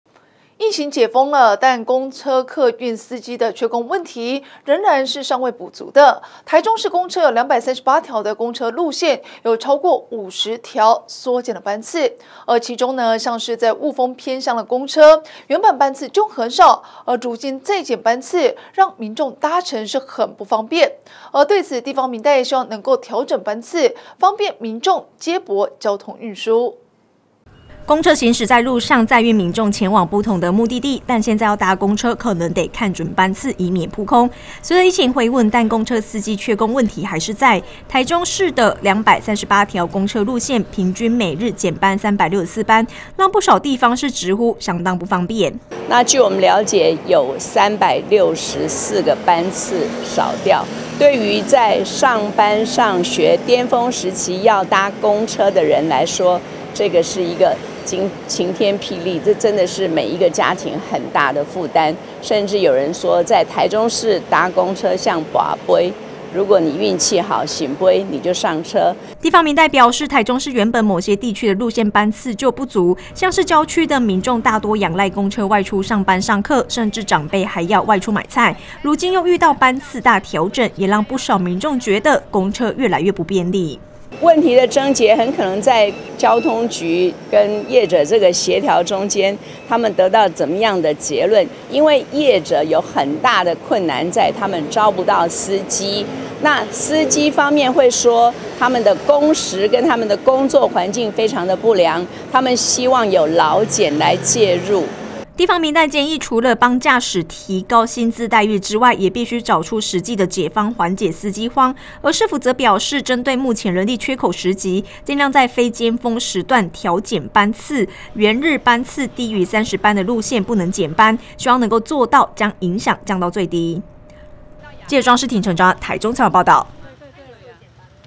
公車人力荒大減班 偏鄉公車不易等─大台中新聞
隨著疫情解封，但公車客運司機的缺工問題還是在，為此台中市公車238條公車路線超過50條縮減班次，但其中像是霧峰偏鄉的公車，原本班次就很少，如今再減班次讓民眾搭乘更不方便，對此地方民代希望能夠調整班次，方便民眾接駁交通運輸。「以上新聞可由top NEWS APP收看」